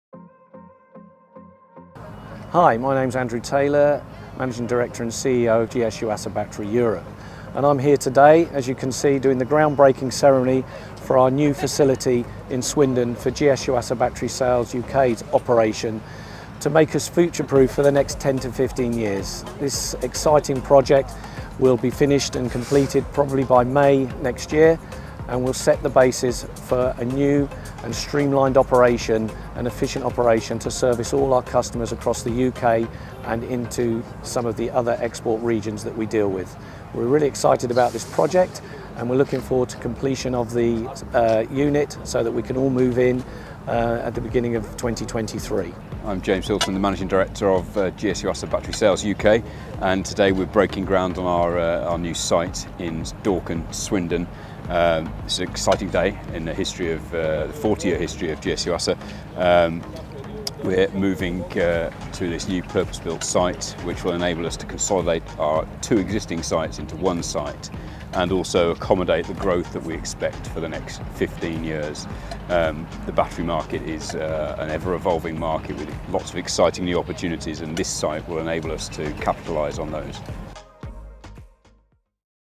GS Yuasa New Site Ground Breaking Ceremony
We have begun construction of our new UK head office and distribution centre Dorcan, Swindon.